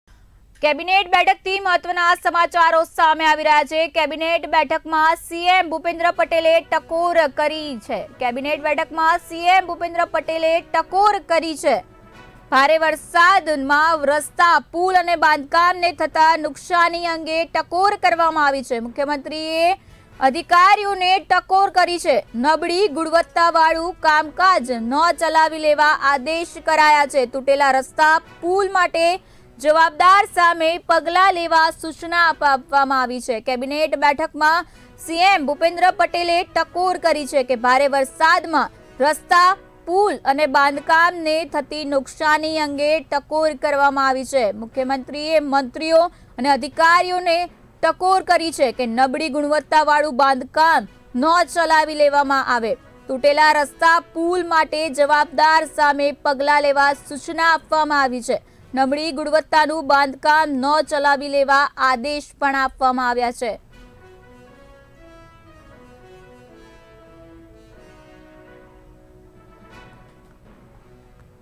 કેબિનેટ બેઠકમાં CM ભૂપેન્દ્ર પટેલે કરી ટકોર
ભારે વરસાદમાં રસ્તા,પુલ અને બાંધકામને થતી નુકશાની અંગે કરી ટકોર
મુખ્યમંત્રીએ મંત્રીઓ અને અધિકારીઓને કરી ટકોર
નબળી ગુણવત્તાવાળું બાંધકામ ન ચાલવી લેવા આદેશ
તૂટેલા રસ્ત્તા,પુલ માટે જવાબદાર સામે પગલા લેવા સુચના